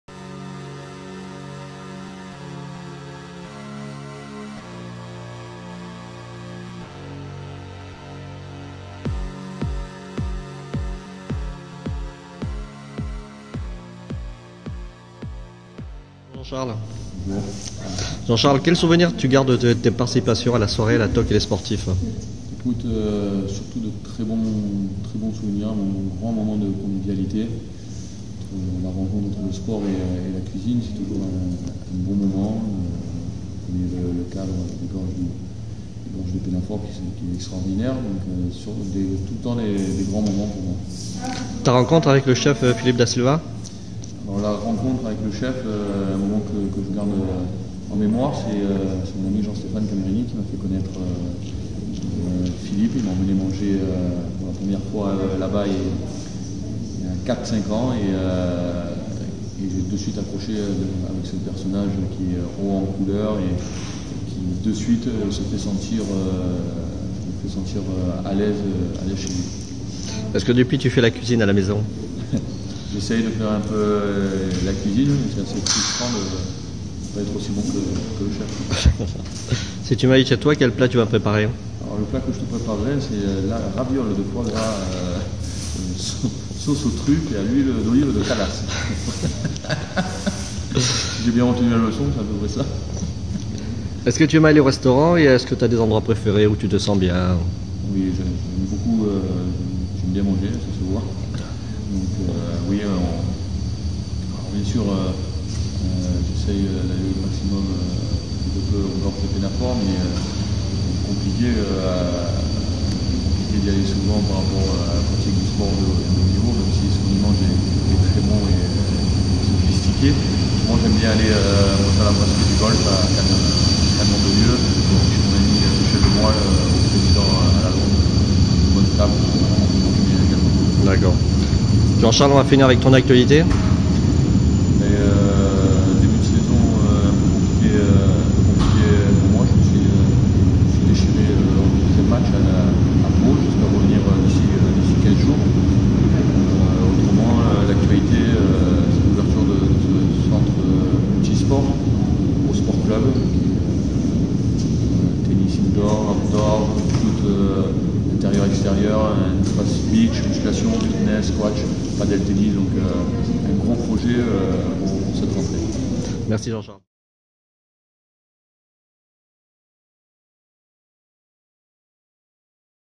LA TOQUE ET LES SPORTIFS A la rencontre de Jean Charles ORIOLI
ITV de jean charles Orioli qui à participé à la 17e édition de la soirée, Ambiance, Cuisine, Actualité